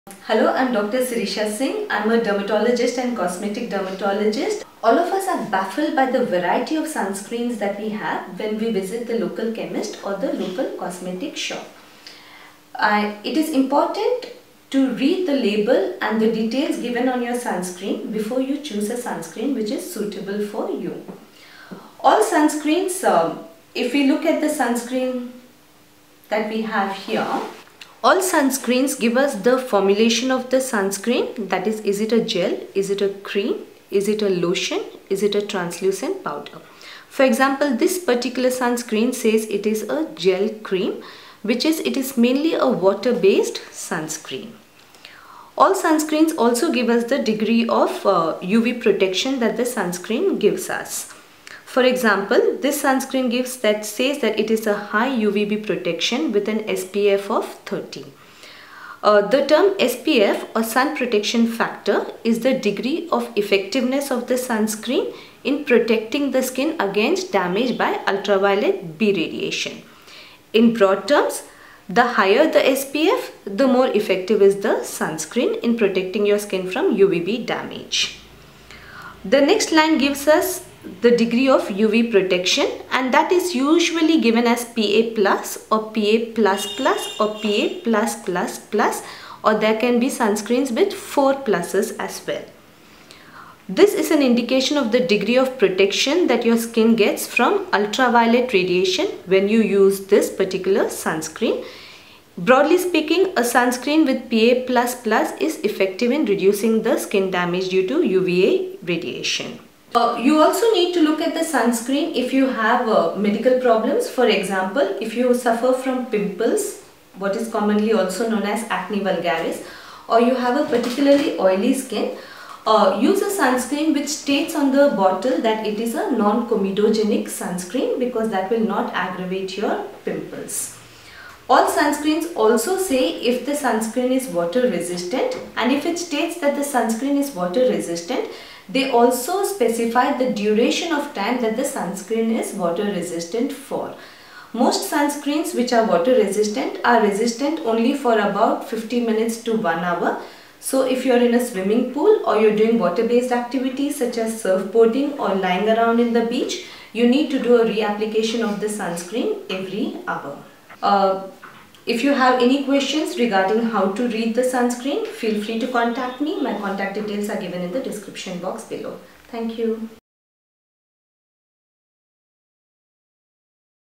Sunscreen Guide - How to Read Information on Sunscreen Bottle
Confused about which sunscreen to buy? Don't worry consultant dermatologist and cosmetic dermatologist Dr Sirisha Singh ) explains how to read the information on the sunscreen bottle. Here are the important takeaways.

1. SPF - This is probably the first thing you should look at before buying the sunscreen. A SPF of 30 is ideal for most.
2. PA Rating - You should look at a PA++ rated sunscreen
3. If you have pimples or oily skin then go for a non comedogenic sunscreen.
4. Water resistance - Buy a water resistant sunscreen if you plan a water activity.